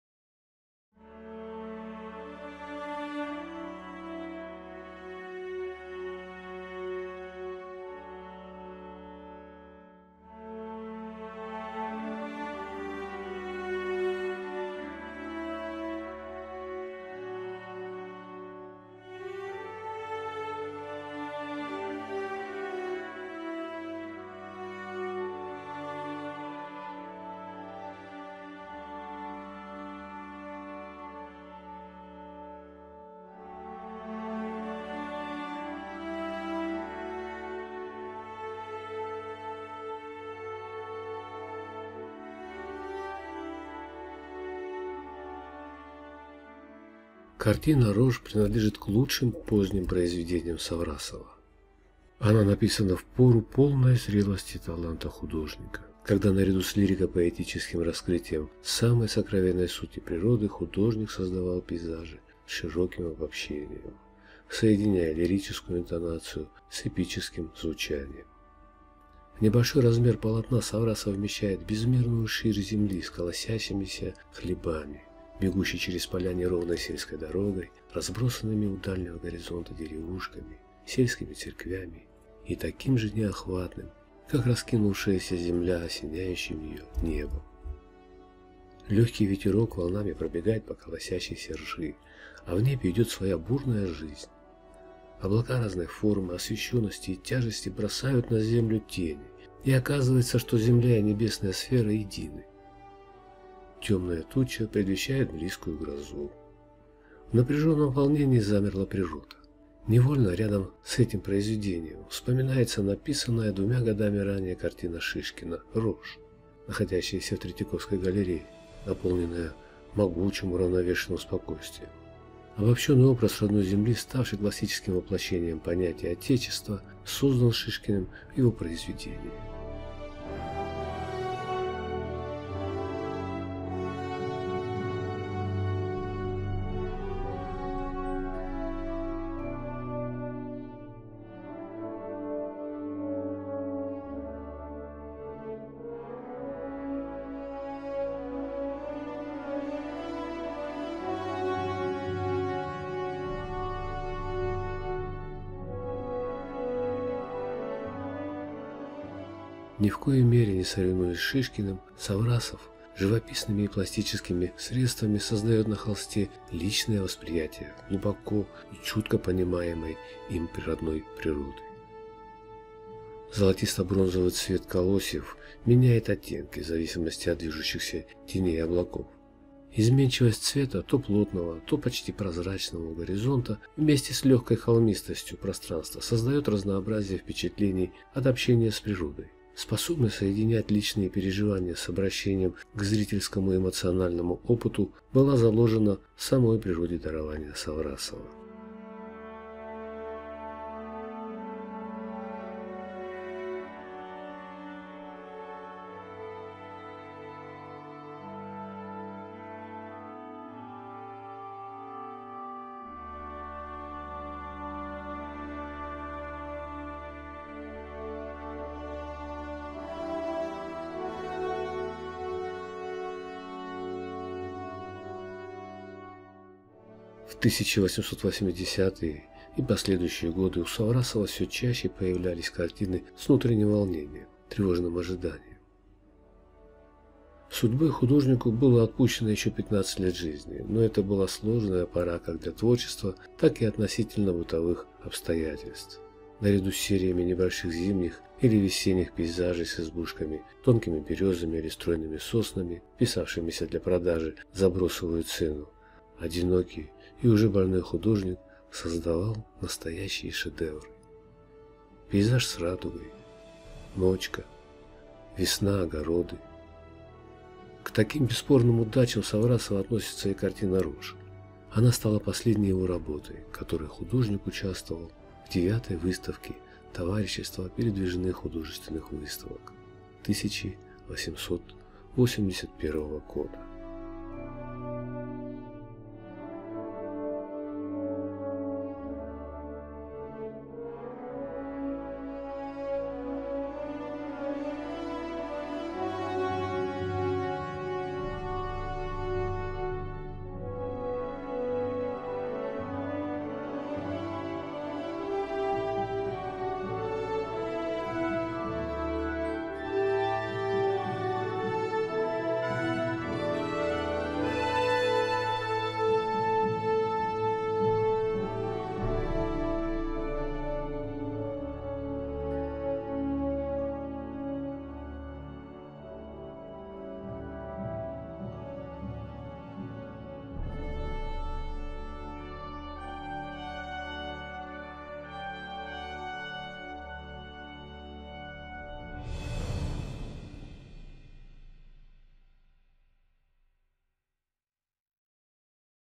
Саврасов Алексей. «Рожь».
Картина «Рожь» написана в то время, когда
талант Саврасова достиг полной зрелости
и принадлежит к его лучшим поздним работам.

Описание картины «Рожь»:
Саврасов Алексей Кондратьевич
Дата 1881
масло на холсте
Размеры
Высота: 45,4 см (17,87 дюйм);
Ширина: 64 см (25,19 дюйма);
Местонахождение картины «Рожь»:
Государственная Третьяковская галерея
Приобретено у С.А. Шлиппе. 1948